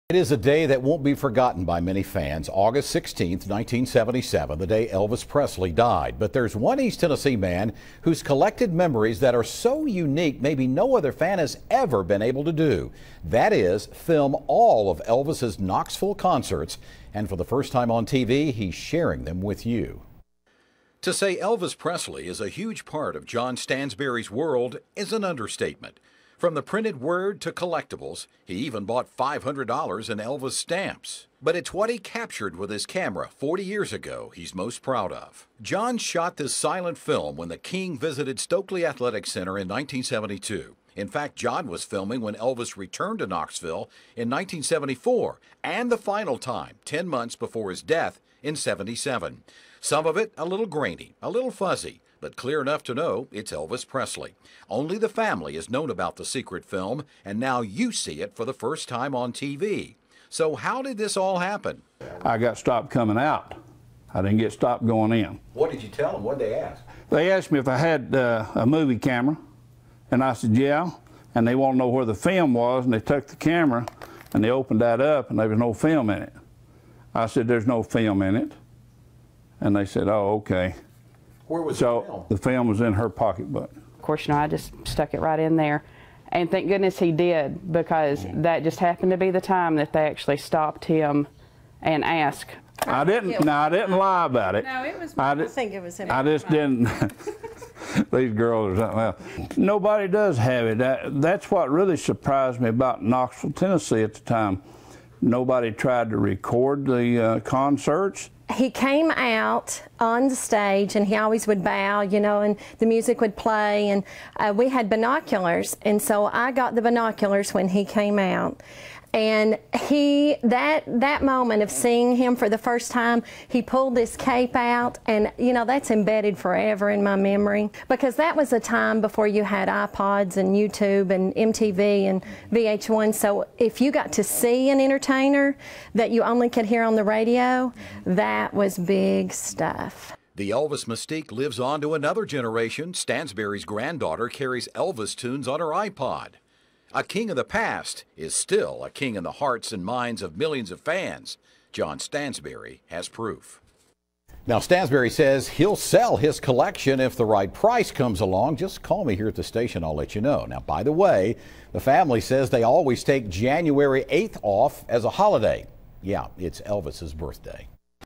Never before seen Elvis video from Knoxville concerts
KNOXVILLE, Tenn.(WVLT)---To say Elvis Presley is a huge part of John Stansberry's world is an understatement.

From newspapers to 500 dollars in postage stamps with the face of Elvis, Stansberry has collected alot of memorabilia.

But it's what he captured with his camera 40 years ago that he's most proud of.

John shot film of Elvis when he visited Stokely Athletic Center in 1972, 1974, and 10 months before Elvis died in 1977.

The film is a little grainy, a little fuzzy but clear enough to know it's Elvis Presley.

Only the family has known about the secret film until now.

John Told Local 8 News Anchor Alan Williams, "I got stopped coming out, I didn't get stopped going in, what did they ask? They asked if I had a movie camera, I said yes, they wanted to know where the film was and they took the camera and opened it up and there was no film in it. It was in my daughter's pocket book."
"I didn;t lie about it!."

Stansberry's daughter, Melissa was beside herself.
"He came out on stage, he always would bow and the music would play we had binoculars and I got the binoculars
that moment of seeing him for the first time he pulled his cape out, that's embedded in my memory."

"That was a time before you had Ipods, You tube, MTV, and VH1 if you got to see an entertainer that you only could hear on the radio, that was big stuff."

A king of the past is still king in the hearts and minds of millions of fans. John Stansberry has proof.